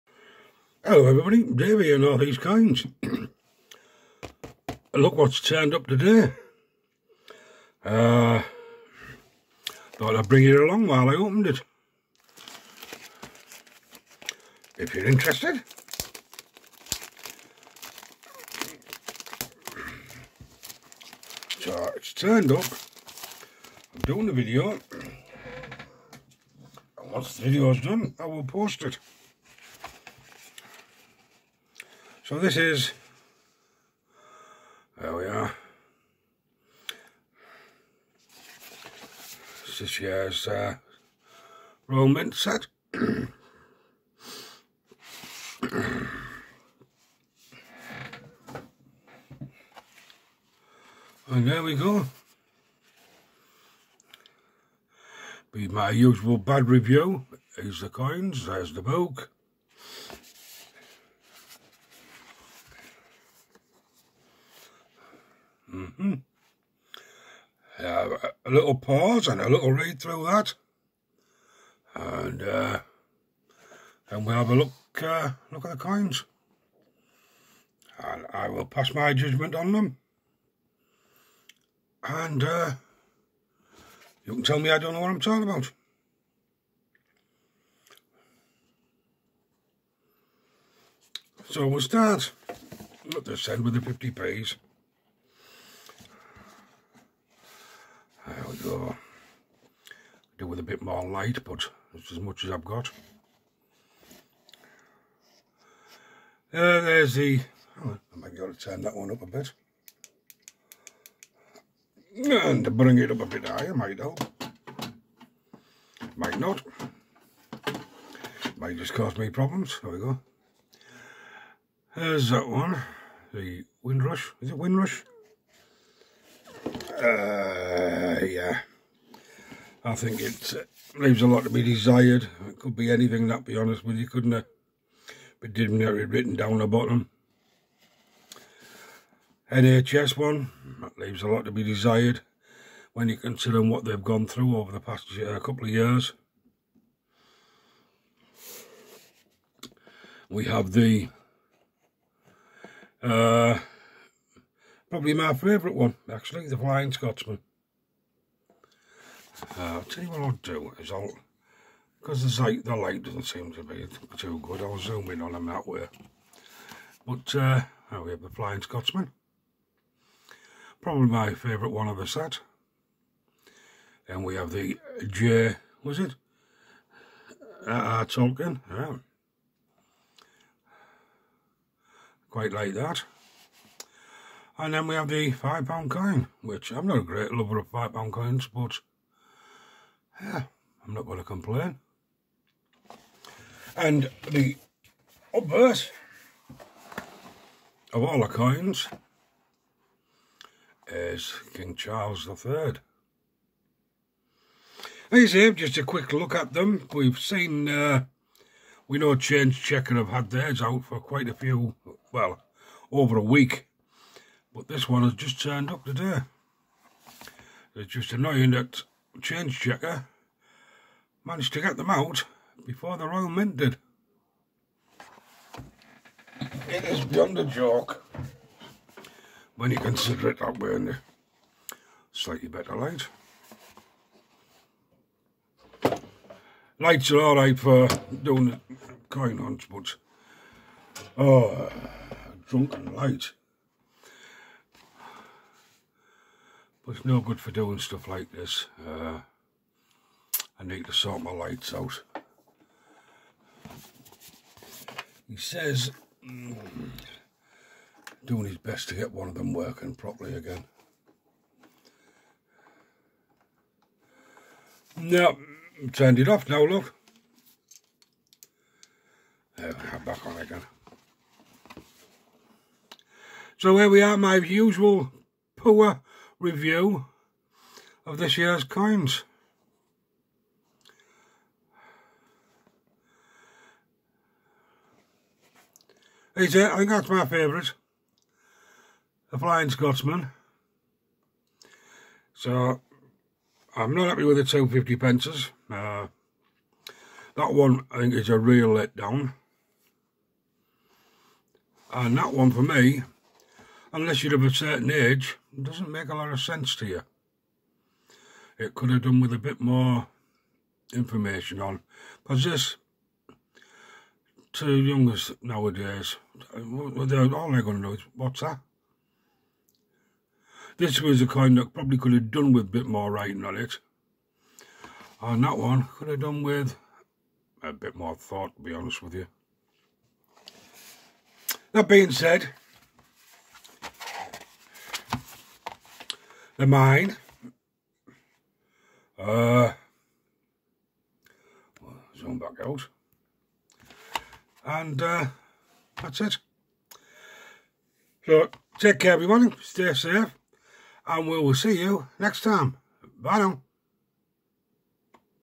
What's on Dave's Desk Midweek Special #5
Another bad review on this years royal mint annual set for 2023